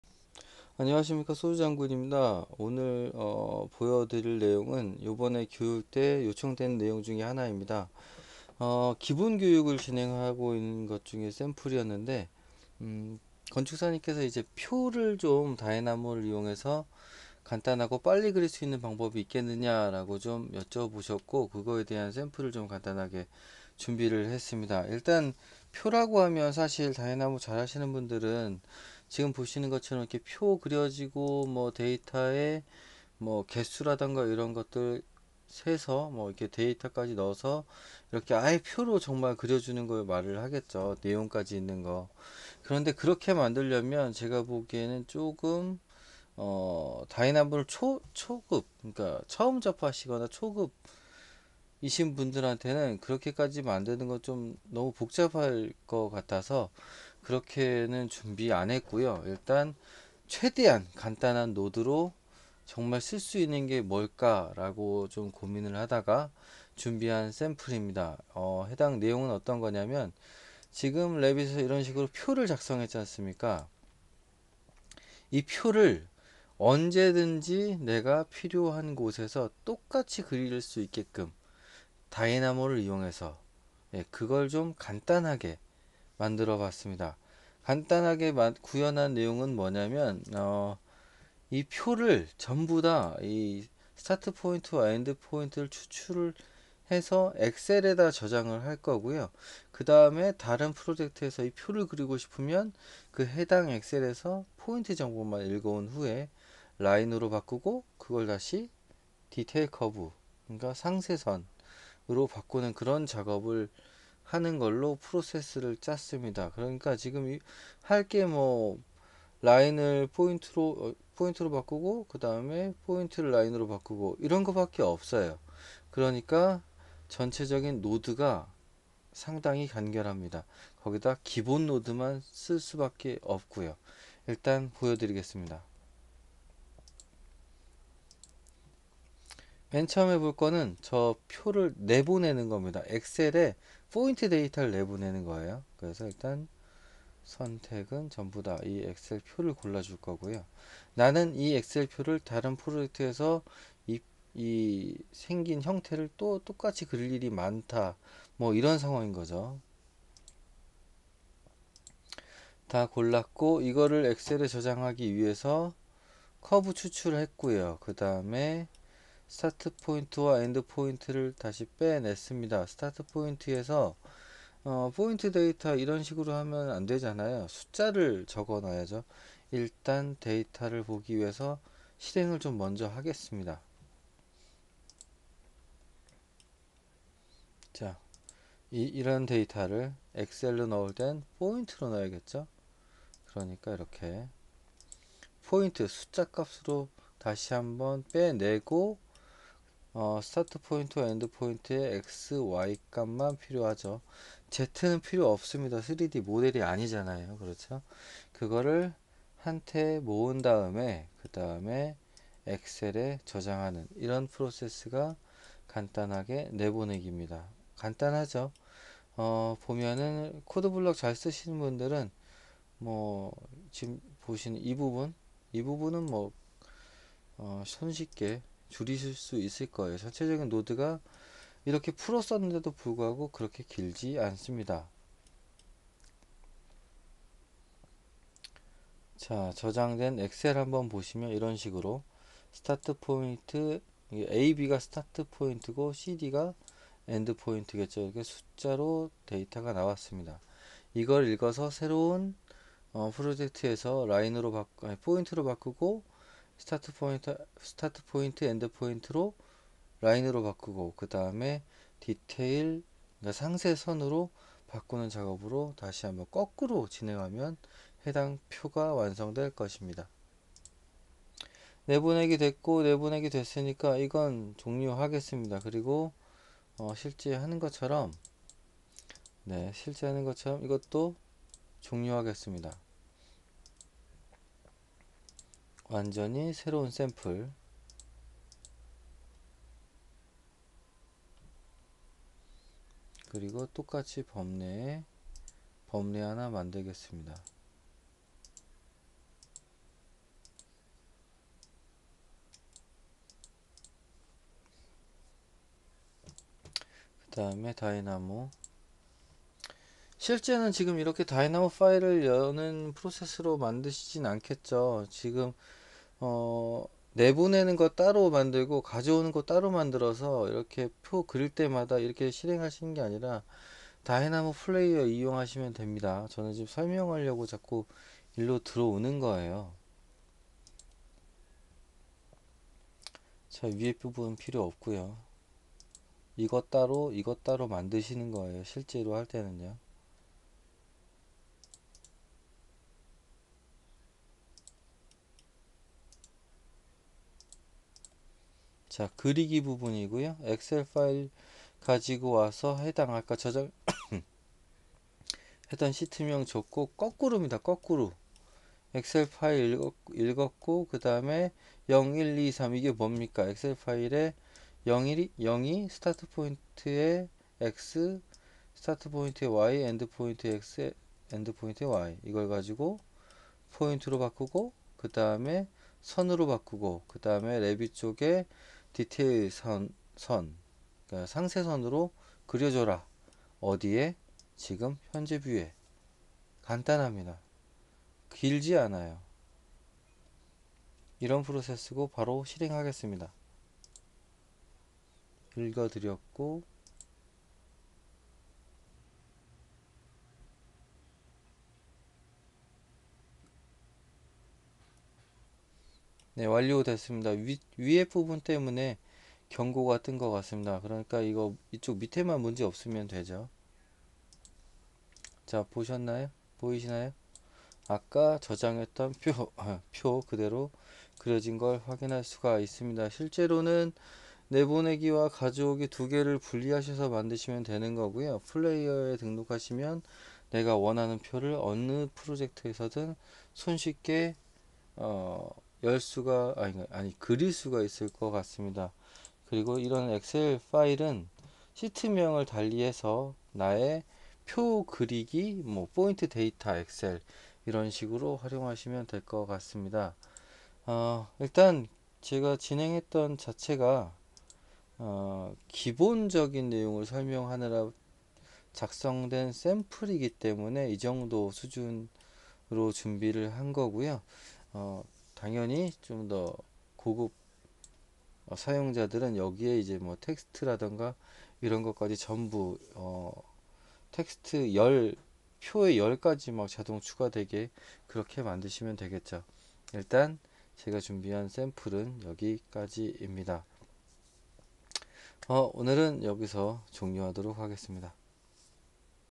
엑셀을 이용한 사용자 표 만들기 샘플
교육시 요청되었던 샘플입니다. 간단한 노드로 엑셀을 이용해서 표를 작성하는 내용입니다.